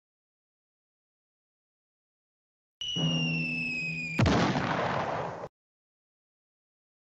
Funniest 5 Second Fall - TURN UP YOUR SOUND!- Real Life Wile E. Coyote.
I can't watch this without laughing my butt off. Enjoy!  Not as funny without sound (I added the effect myself).